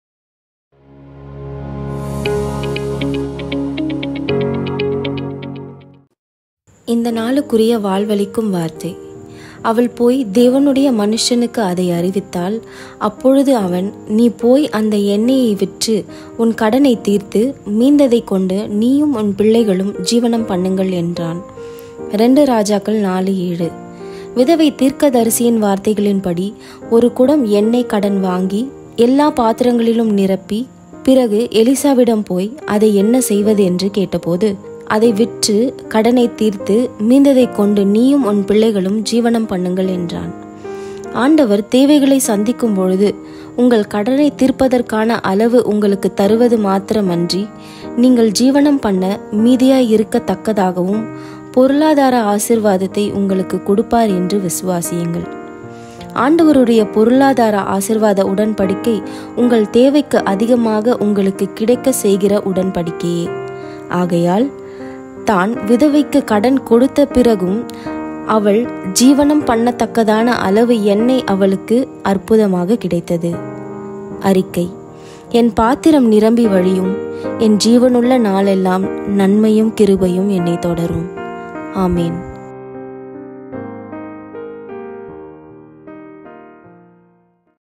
இந்த நாளுக்குரிய "வாழ்வளிக்கும் வார்த்தை" | Vaazhvalikkum Vaarthai | Vaarthai TV
இந்த நாளுக்குரிய "வாழ்வளிக்கும் வார்த்தை"

செவ்வாய்க்கிழமை 23/08/2022

அவள் போய் தேவனுடைய மனுஷனுக்கு அதை அறிவித்தாள். அப்பொழுது அவன்: நீ போய் அந்த எண்ணெயை விற்று, உன் கடனைத் தீர்த்து, மீந்ததைக்கொண்டு நீயும் உன் பிள்ளைகளும் ஜீவனம் பண்ணுங்கள் என்றான். 2 இராஜாக்கள் 4:7

விதவை தீர்க்கதரிசியின் வார்த்தைகளின் படி ஒரு குடம் எண்ணெய் கடன் வாங்கி எல்லா பாத்திரங்களிலும் நிரப்பி பிறகு எலிசாவிடம் போய் அதை என்ன செய்வது என்று கேட்டபோது. அதை விற்று கடனை தீர்த்து மீந்ததைக் கொண்டு நீயும் உன் பிள்ளைகளும் ஜீவனம் பண்ணுங்கள் என்றான். ஆண்டவர் தேவைகளை சந்திக்கும் போது உங்கள் கடனை தீர்ப்பதற்கான அளவு உங்களுக்கு தருவது மாத்திரமன்றி நீங்கள் ஜீவனம் பண்ண மீதியாயிருக்கத்தக்கதாகவும் பொருளாதார ஆசீர்வாதத்தை உங்களுக்கு கொடுப்பார் என்று விசுவாசியுங்கள். ஆண்டவருடைய பொருளாதார ஆசீர்வாத உடன்படிக்கை உங்கள் தேவைக்கும் அதிகமாக உங்களுக்கு கிடைக்கச் செய்கிற உடன்படிக்கையே. ஆகையால் தான் விதவைக்கு கடனை கொடுத்த பிறகும் அவள் ஜீவனும் பண்ணத்தக்கதான அளவு எண்ணெய் அவளுக்கு அற்புதமாக கிடைத்தது.

அறிக்கை:
என் பாத்திரம் நிரம்பி வழியும். என் ஜீவனில் நாளெல்லாம் நன்மையும் கிருபையும் என்னை தொடரும். ஆமென்

தொடர்புக்கு: போதகர் ஆரோன். ரேமா அசெம்பிளி. செங்கல்பட்டு.